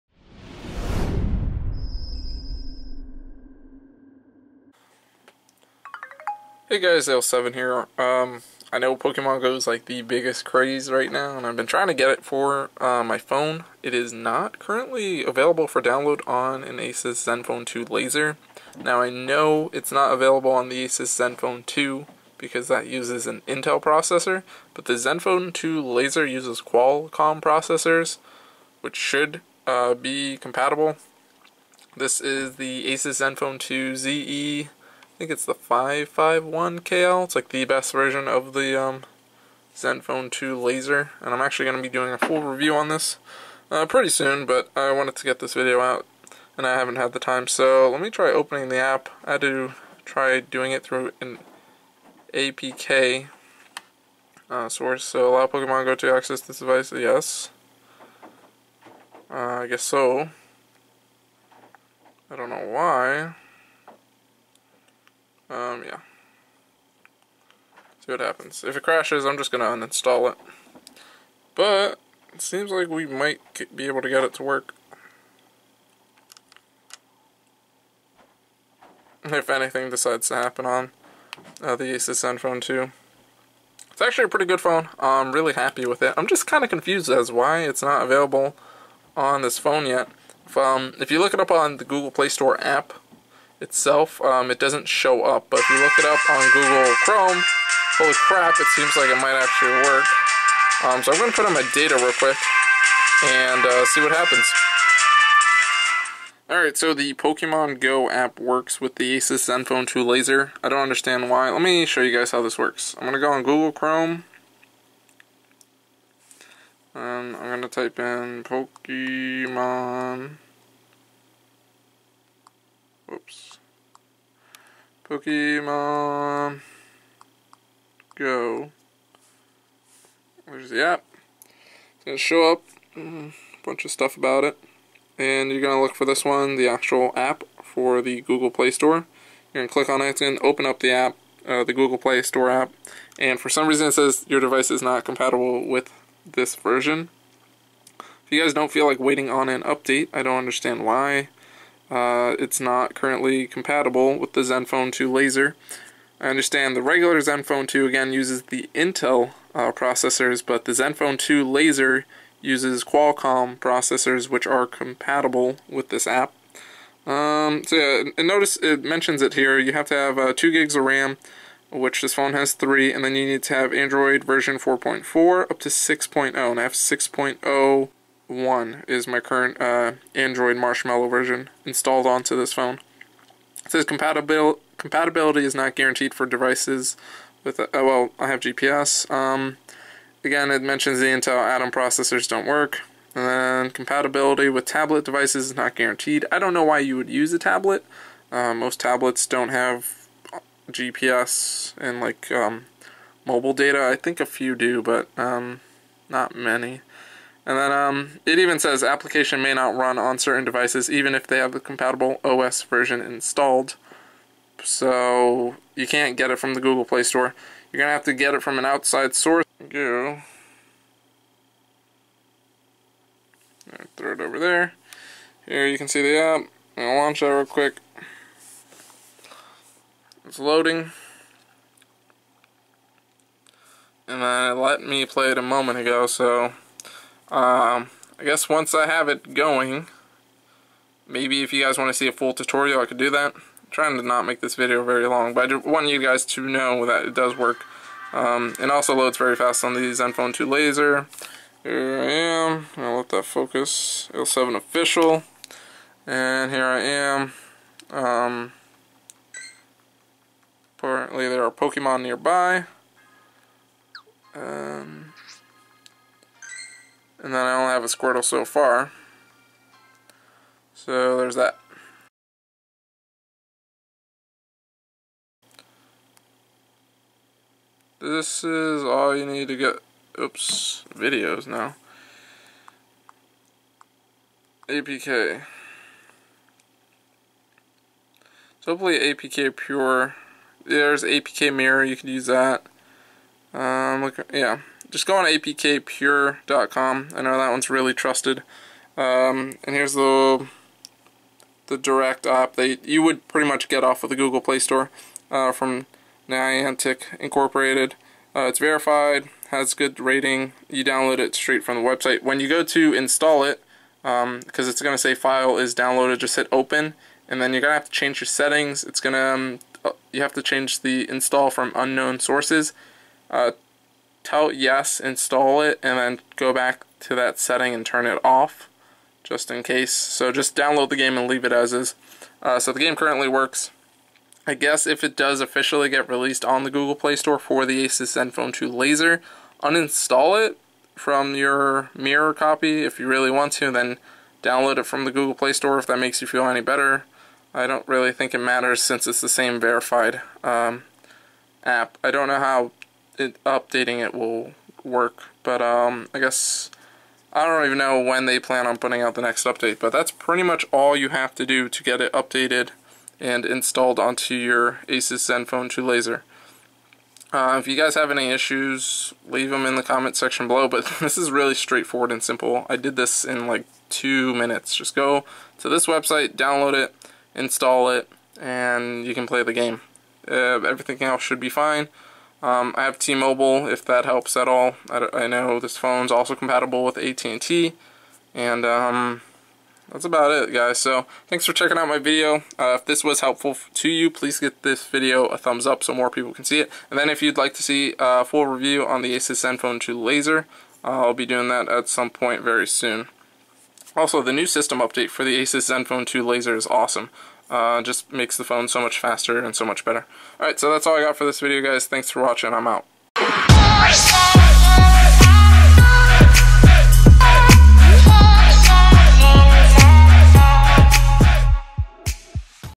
Install Pokemon Go on the ASUS ZenFone 2 Laser
►► Remember to select 1080p 60FPS HD◄◄
The ASUS ZenFone 2 Laser meets all of the hardware and software requirements for Pokemon Go yet isn't available for download on the device.  In this video you'll learn how to download and install Pokemon Go on the ASUS ZenFone 2 Laser!  This will also work on other android phones that can't download the game from the Google Playstore.